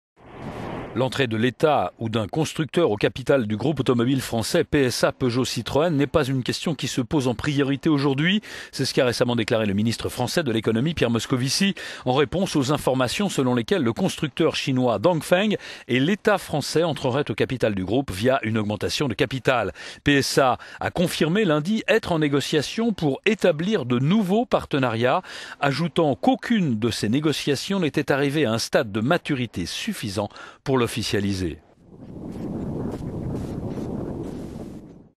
PSA Peugeot Citroën pourrait faire entrer le chinois Dongfeng et l'Etat français à son... - economy
L'entrée de l'Etat ou d'un constructeur au capital du groupe automobile français PSA Peugeot...

L'entrée de l'Etat ou d'un constructeur au capital du groupe automobile français PSA Peugeot Citroën n'est pas une question qui se pose en priorité aujourd'hui, c'est ce qu'a récemment déclaré le ministre français de l'Economie Pierre Moscovici, en réponse aux informations selon lesquelles le constructeur chinois Dongfeng et de l'Etat français entrerait au capital du groupe via une augmentation de capital. PSA a confirmé lundi être en négociations pour établir de  nouveaux partenariats, ajoutant qu'aucune d'entre elle  n'était arrivé à un stade de maturité suffisant pour l'officialiser.